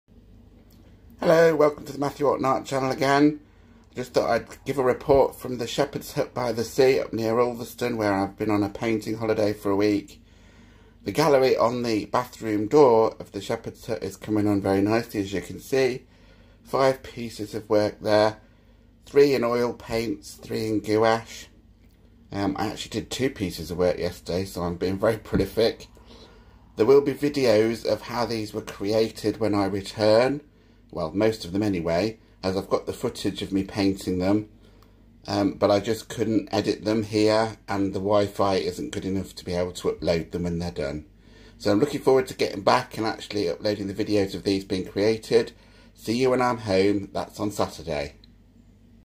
Shepherd's Hut by the Sea Pop up Art Gallery
Latest report from my Shepherd's Hut by the Sea painting holiday. The week away is drawing to a close...one more full day to go...but the pop up art gallery on the bathroom door of the hut is looking great with six paintings now...three in gouache and three in oil paints.